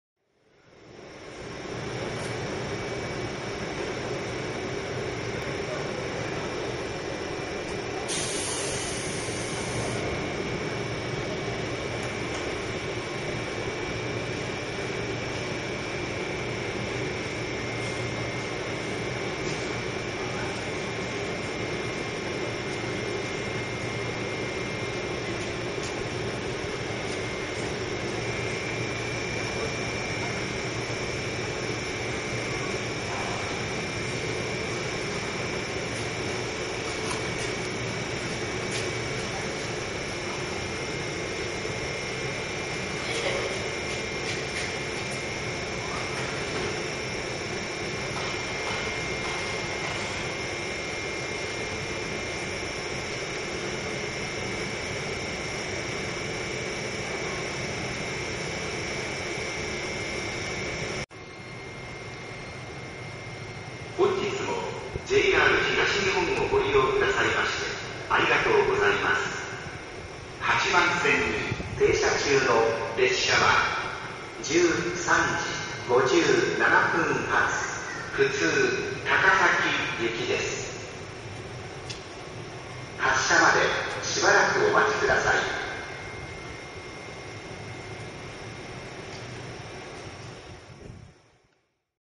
両毛線の小山発池袋行き！？
両毛線の小山駅に池袋幕の２１１系が！？
高崎回りで向かうのか？　気が遠くなります・・・。

なんてことはなく、やっぱり間違いだったみたいで、
後で方向幕を回転さてていました。（結局いつも通りの高崎行き。）

ちなみに小山駅から湘南新宿ラインで池袋に行けば６３分のところ、
このルートだと乗り換え時間を考えないで３時間３８分かかります・・・。